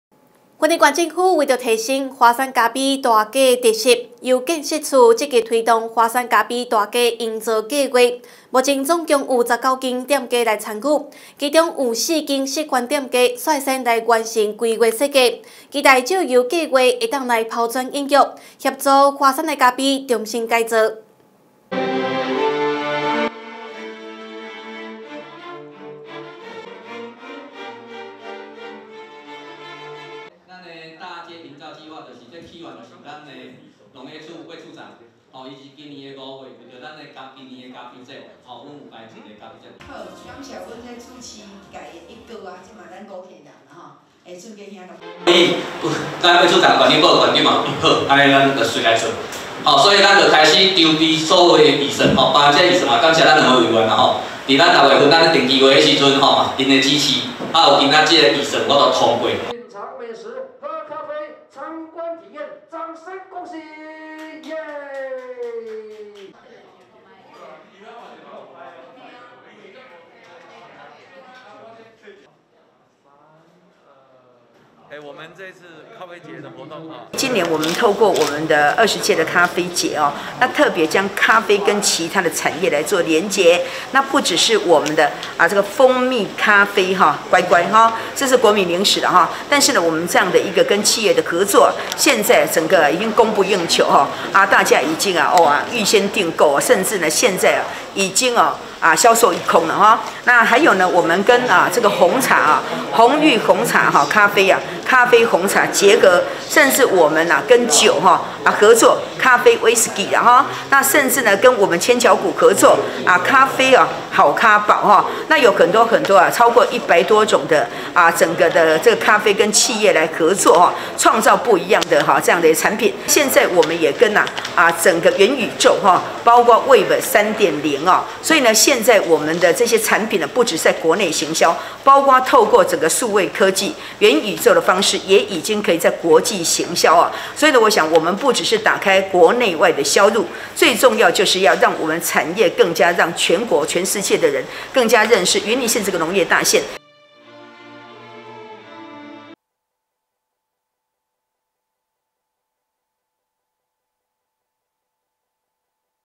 改頭換面！ 雲林古坑華山咖啡大街啟動營造計畫-雲林新聞網
雲林縣政府為提升華山咖啡大街獨特性與記憶點，由建設處積極推動「華山咖啡大街營造計畫」，目前共有19間商家參與，其中有4間示範店家率先完成規劃設計，期待藉由計畫能拋磚引玉，協助華山咖啡重新改造，進而提供遊客更優質的服務。